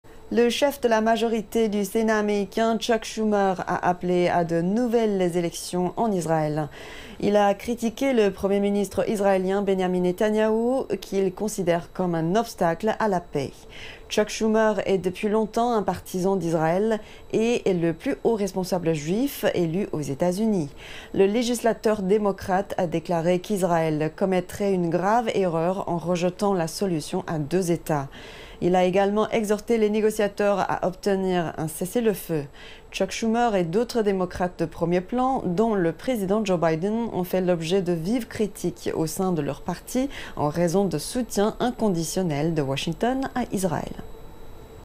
Le chef de la majorité au Sénat américain appelle à de nouvelles élections en Israël
Le chef de la majorité du Sénat américain, Chuck Schumer, a appelé jeudi à de nouvelles élections en Israël. Il a critiqué le Premier ministre israélien Benyamin Netanyahou, qu'il considère comme un obstacle à la paix. Chuck Schumer est depuis longtemps un partisan d'Israël et est le plus haut responsable juif élu aux États-Unis. Le législateur démocrate a déclaré qu'Israël commettrait une "grave erreur" en rejetant la solution à deux États. Il a également exhorté les négociateurs à obtenir un cessez-le-feu. Chuck Schumer et d'autres démocrates de premier plan, dont le président Joe Biden, ont fait l'objet de vives critiques au sein de leur parti en raison du soutien inconditionnel de Washington à Israël.